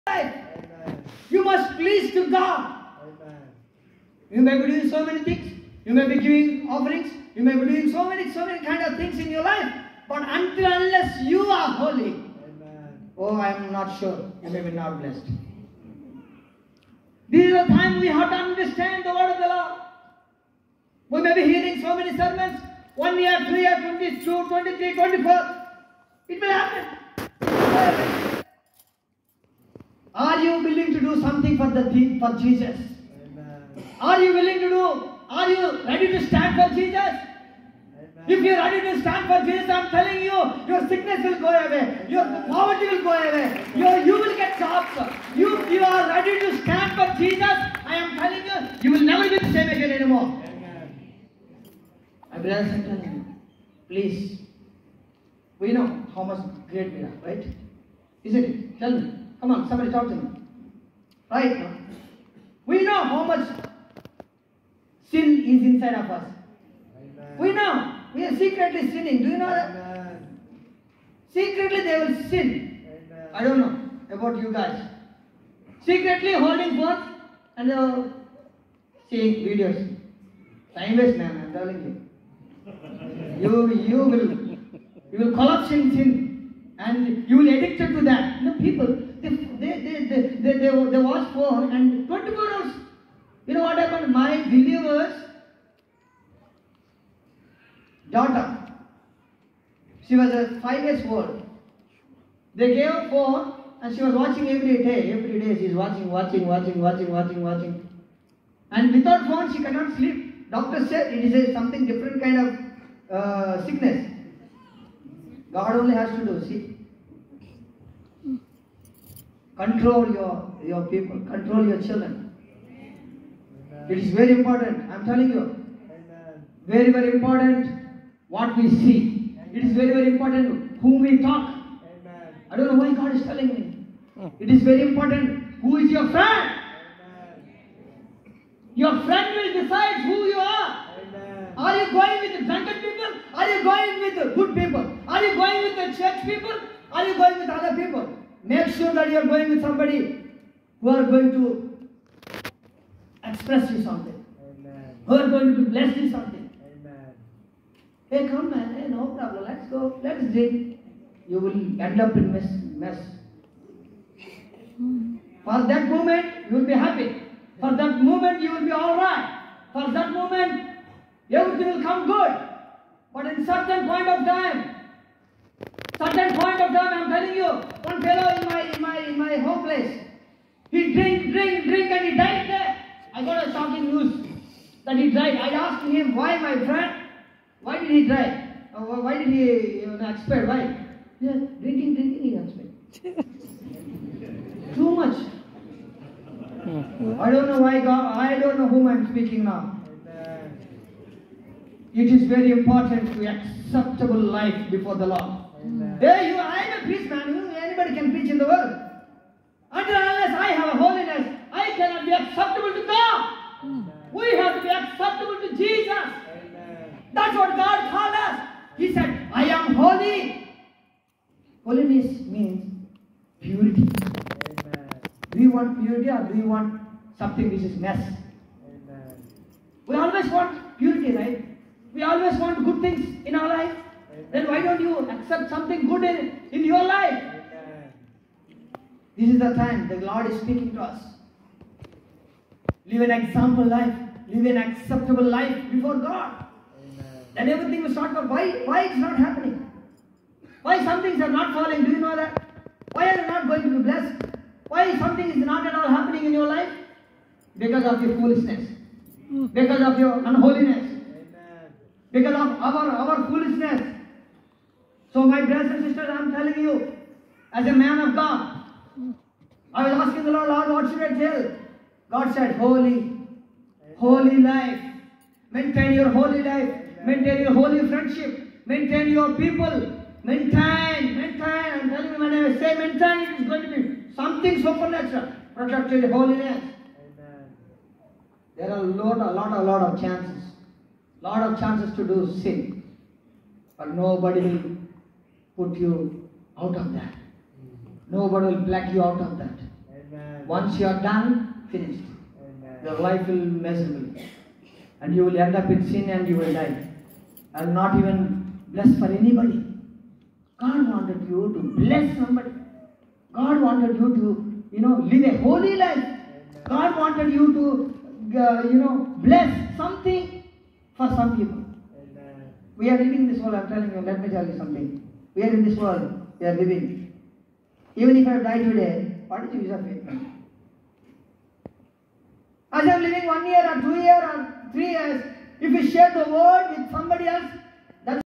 ps krupa rakshana sharing about holiness in Durban south Africa
Video from Rev Krupa Rakshana